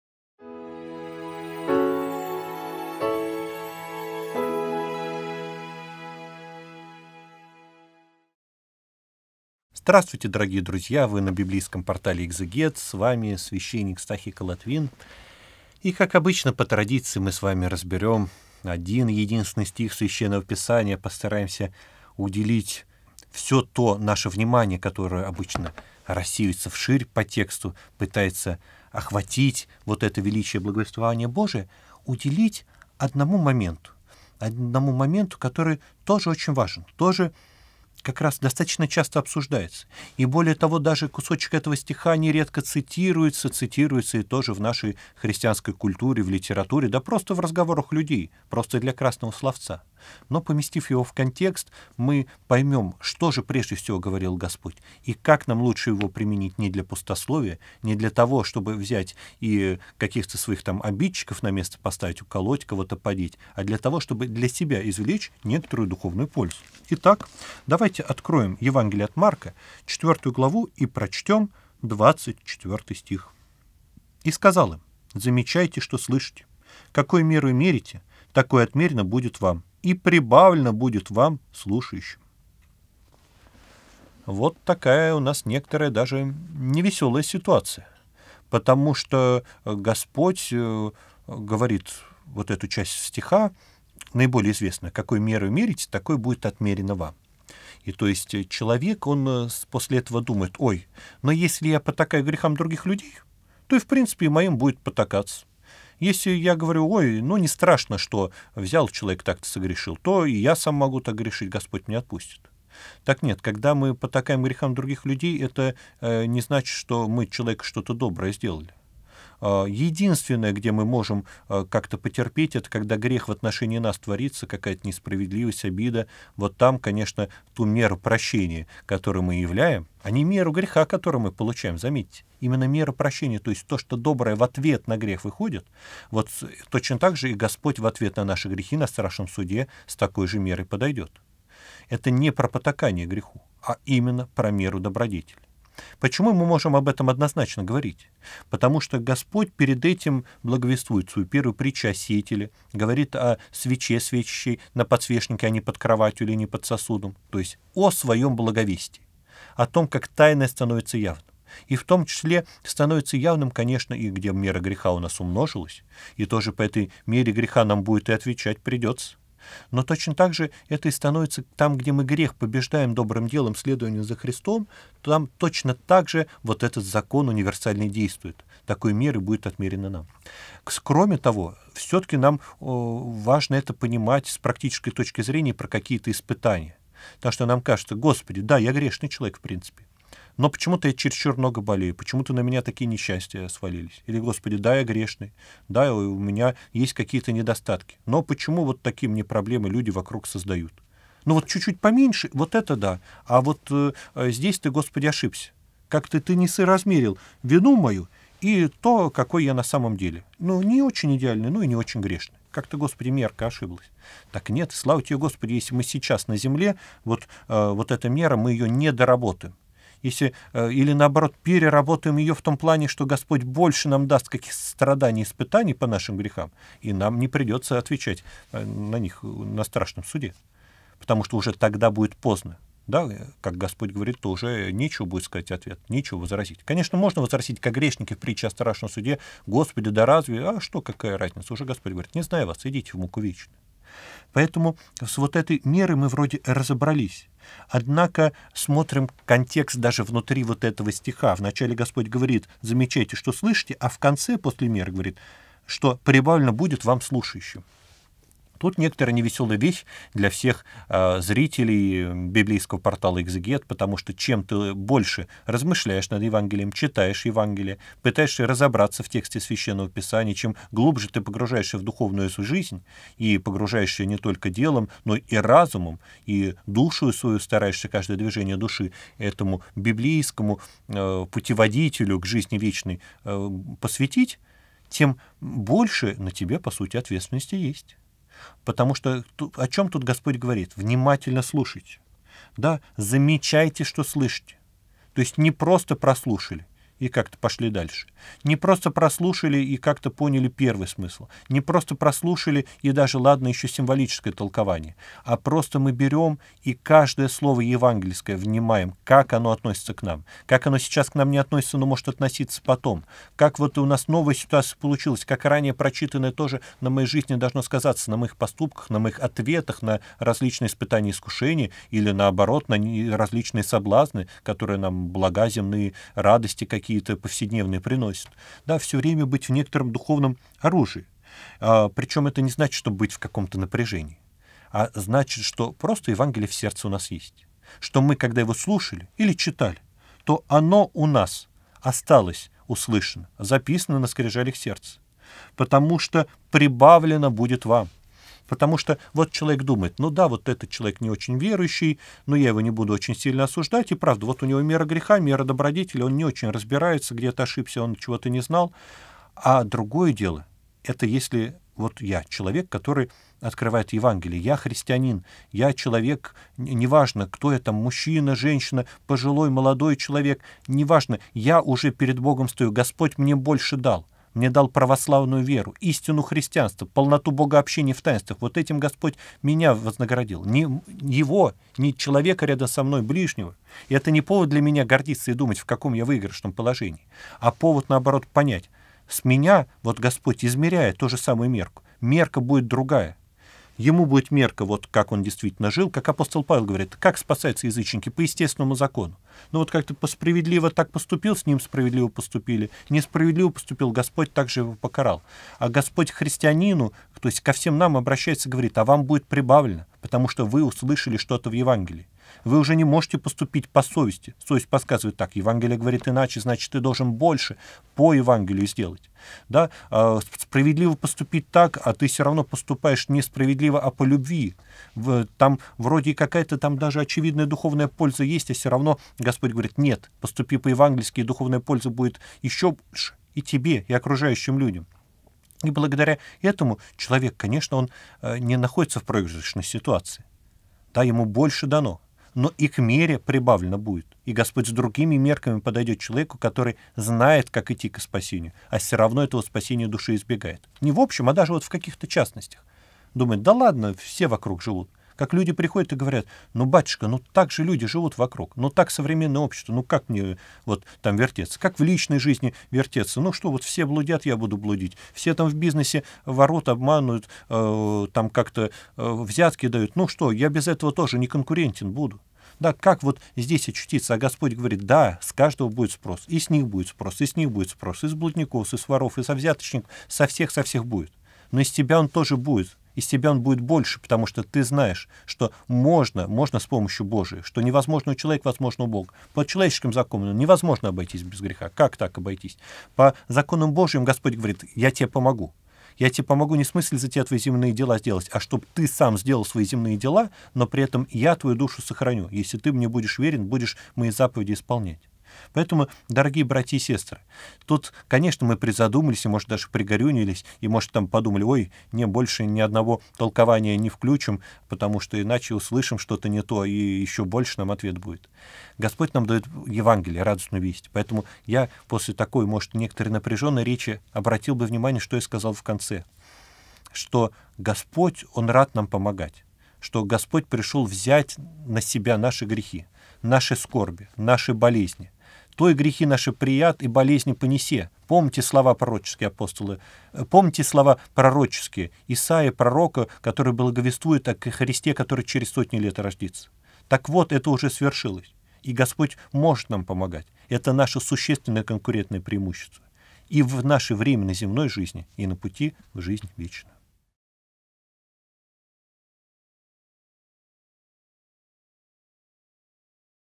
"Какою мерою мерите, такою отмерено будет вам" (Мк. 4:24). Священник Стахий Колотвин
В сегодняшнем видео со священником Стахием Колотвиным разберем  24-й стих из 4 главы Евангелия от Марка: "И сказал им: замечайте, что слышите: какою мерою мерите, такою отмерено будет вам и прибавлено будет вам, слушающим". Коснемся следующих тем:

🔸 Ежедневные цитаты из Евангелия.
🔸 Мера добродетелей.
🔸 Притча о сеятеле.
🔸 Мера греха.
🔸 Испытание в жизни человека.
🔸 Спрос на Страшном Суде.
🔸 Мудрость Евангельских слов.
🔸 Спасение язычников.
🔸 Спрос с христиан.
🔸 Путь ко спасению души в современном мире.